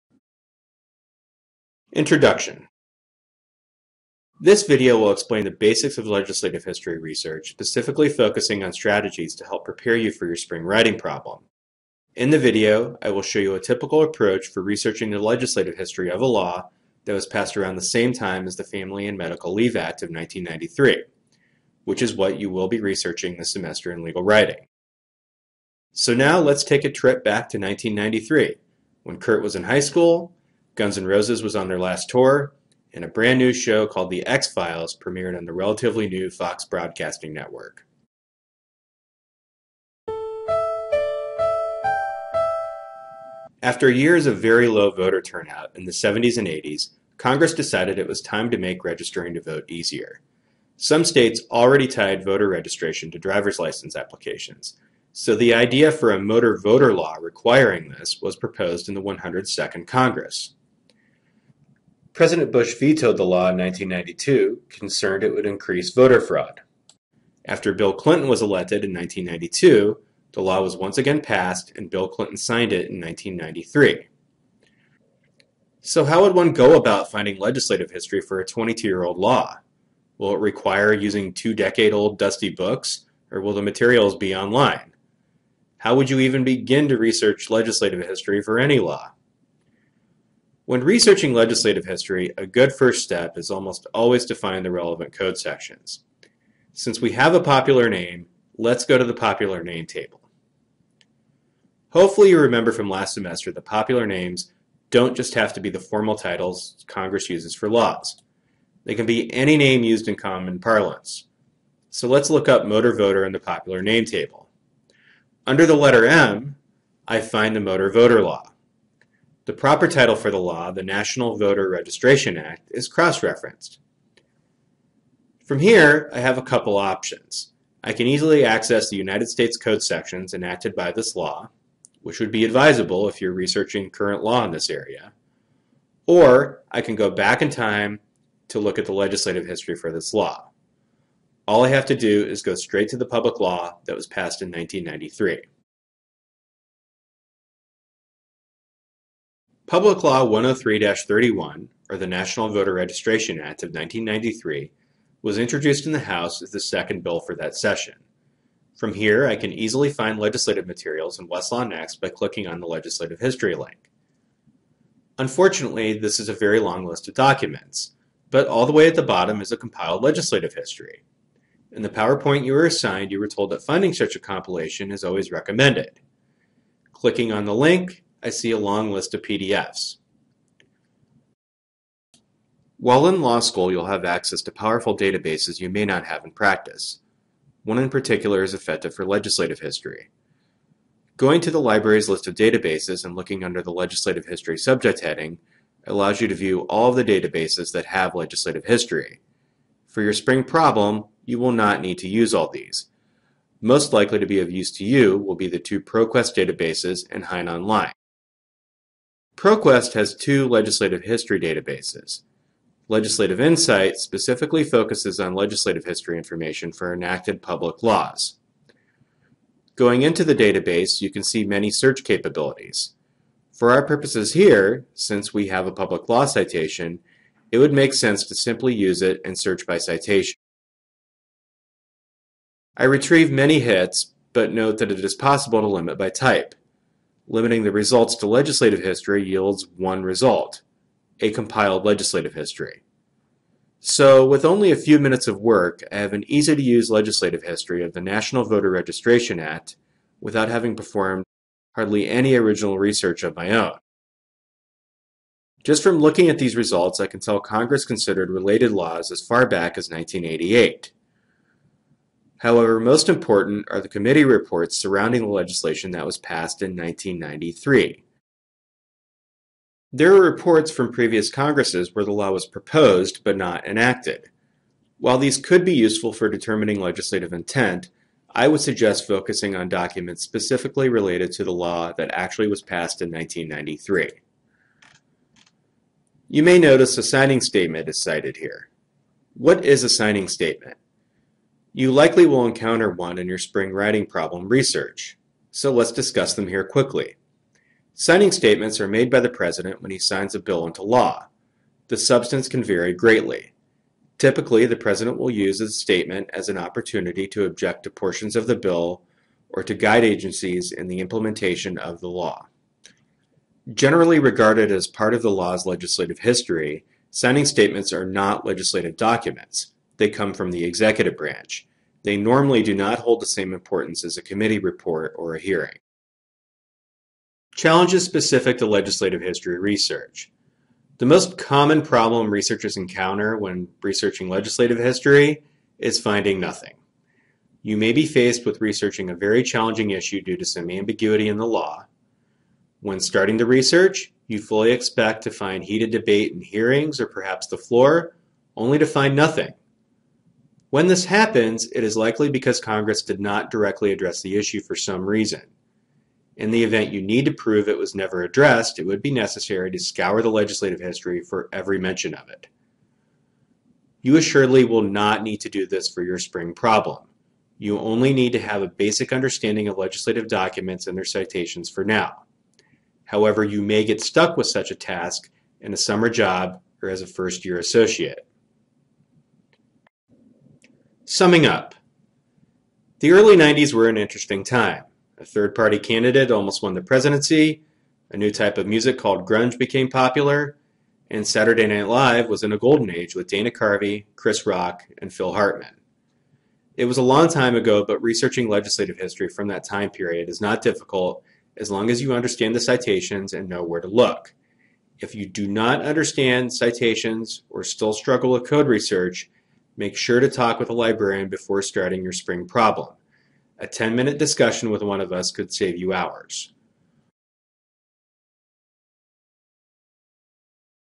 Legislative History Research
This video shows you how to research the legislative history of a law very similar to the one you'll be researching in Legal Writing.